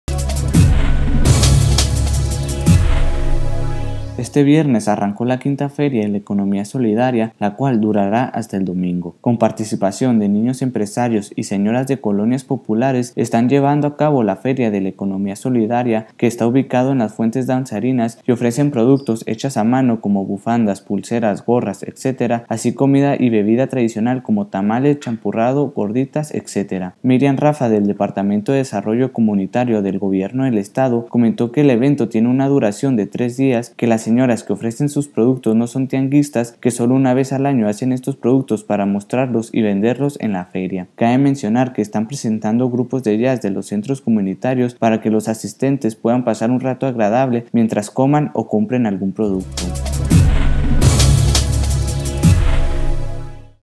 Tiempo TV: Este viernes arrancó la 5ta Feria de la Economía Solidaria
Con participación de niños empresarios y señoras de colonias populares, están llevando a cabo la Feria de la Economía Solidaria, que esta ubicado en las Fuentes Danzarinas, y ofrecen productos echas a mano como bufandas, pulseras, gorras, etc. Así comida y bebida tradicional como tamales, champurrado, gorditas, etc.